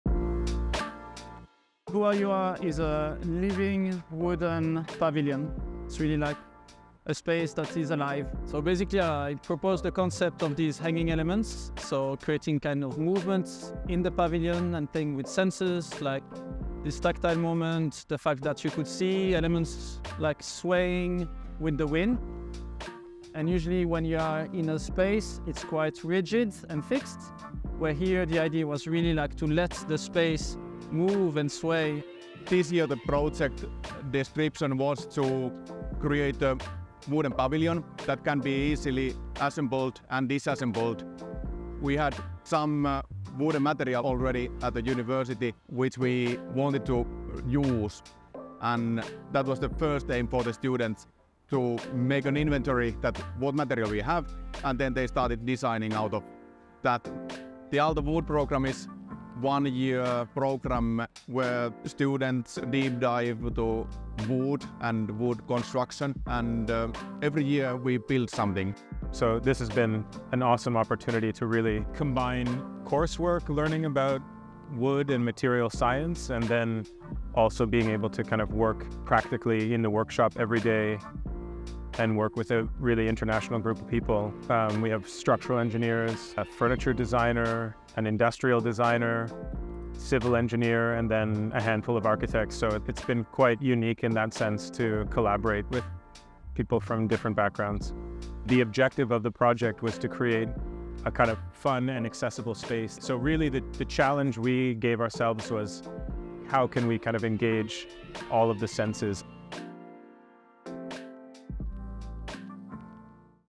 Multi-sensory wood architecture: Huoju-a pavilion
Huoju-a represents a new approach to construction and wood architecture: it is constructed from surplus materials and designed for easy assembly, disassembly, and mobility. This multipurpose structure prioritises ecological sustainability. Its individual components can be easily replaced.

Huoju-a also offers a unique sensory experience. Its hanging wooden elements produce organic sound, creating a serene environment for socialising and relaxation.

Visitors could interact with the structure at the Sideways festival in Helsinki in June 2023 and after that on the Otaniemi campus at Alvar Aallon puisto.

The Aalto Wood Program is a one-year study program that focuses on wood architecture and industrial building. The curriculum is intended for architects, engineers and other students with a background in design and strong interest in timber.

Students:
Adam Smith, Daniel Wolberg, Fraser Plaxton, Ikuyo Tanabe, Kim Gygax, Lennart Huybrechts, Maria Ilina, William Le Gresley, Yi-Hua Wang, Yuki Arima

Instructors:
Laura Zubillaga, Jaakko Torvinen, Pekka Heikkinen & Mauri Konttila